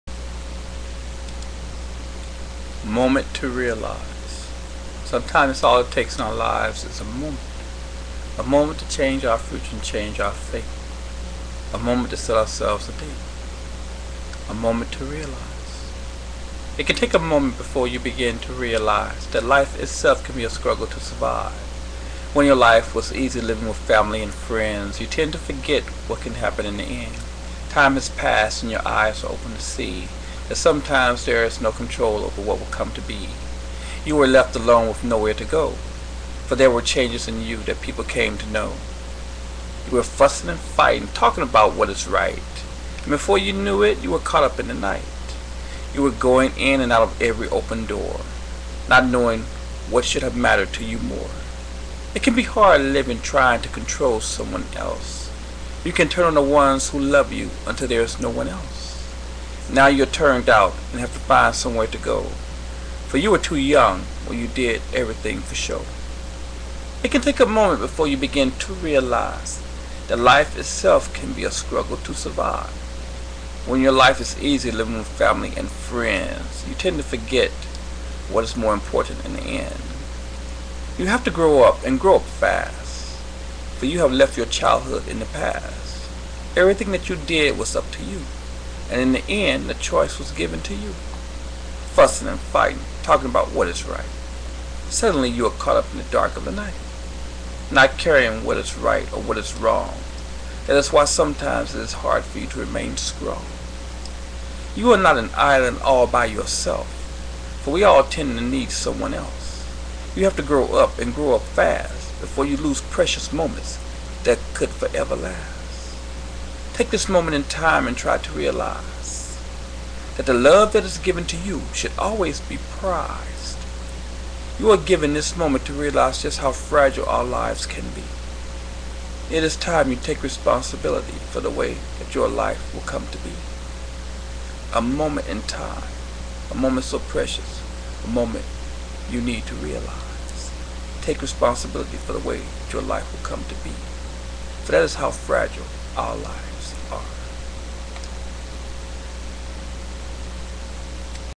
Moment To Realize--Poem
We need to each take a moment in time to understand that we can make our live the way that we want them to be